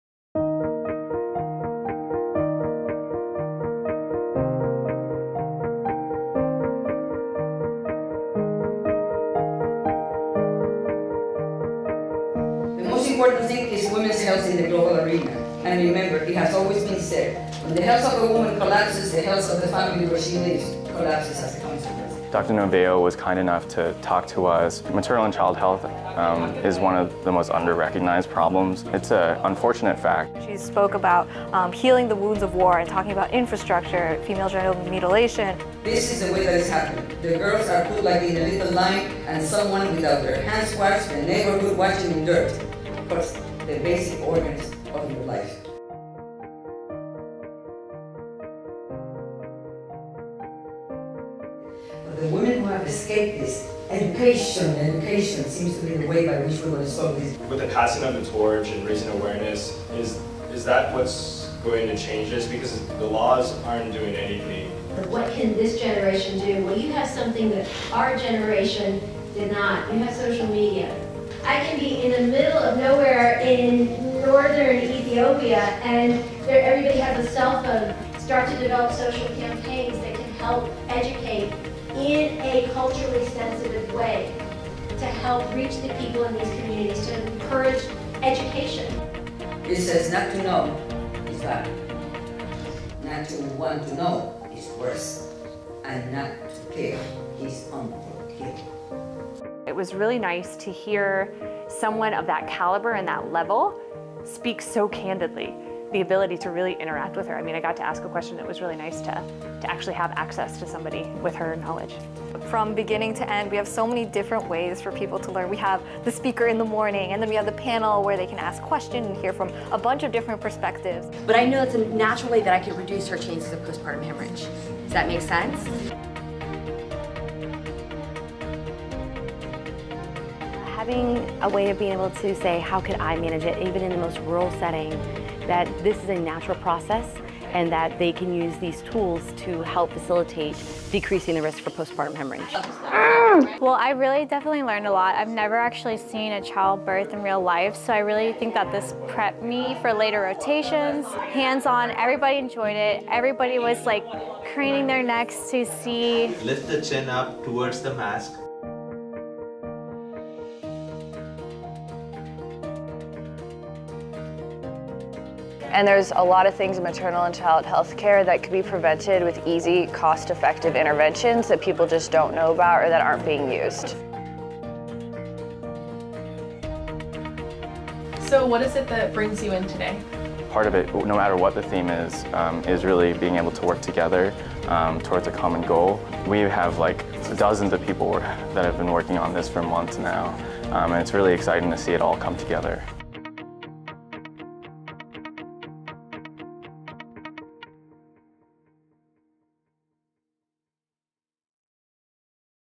UCF Global Health Conference 2015
Nearly 200 medical, pharmacy, nursing and public health students from across Florida spent the day understanding the precarious health of women and babies around the world and how a new generation of healthcare providers can help